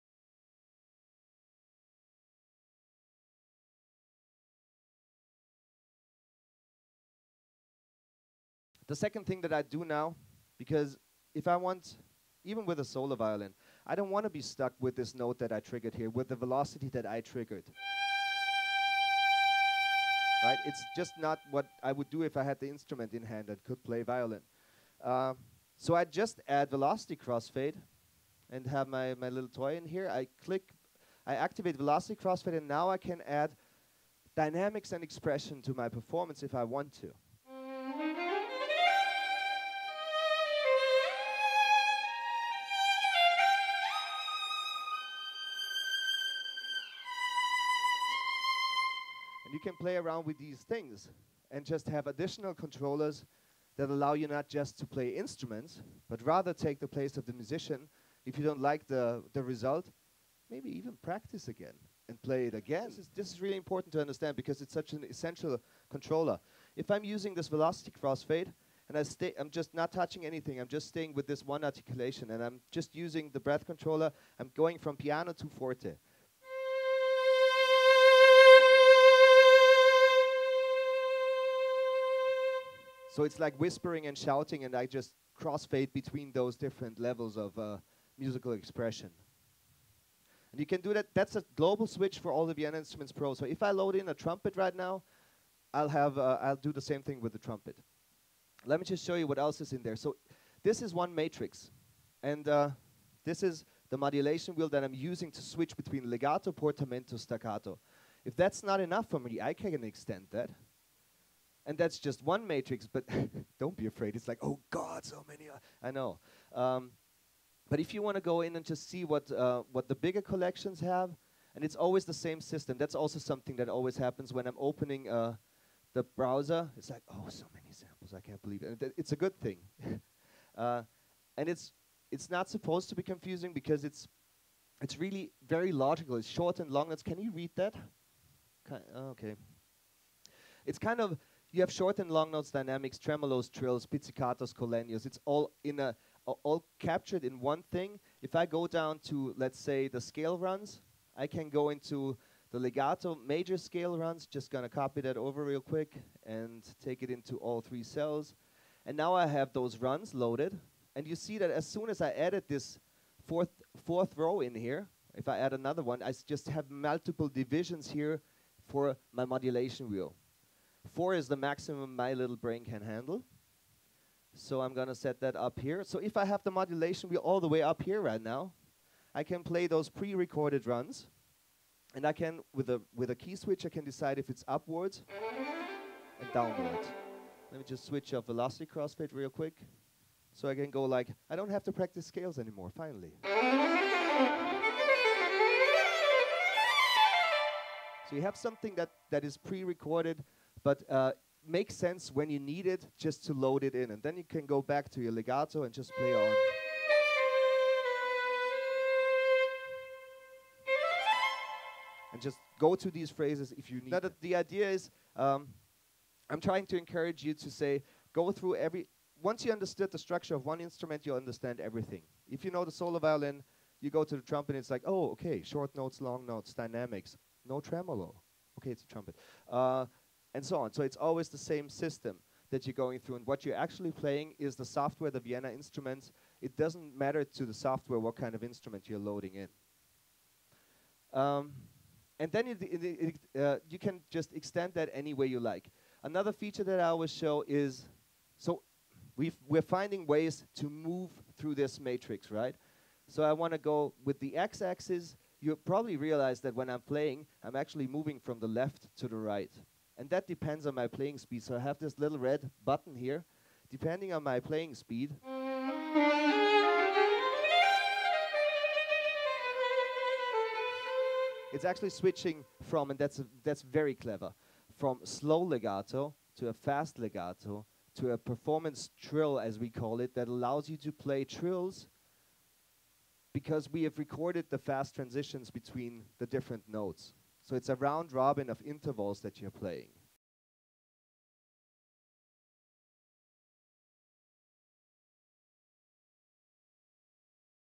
Vienna Instruments with Paul Steinbauer Part 6
Vienna Instruments specialist Paul Steinbauer demonstrates the amazing Vienna Symphonic software.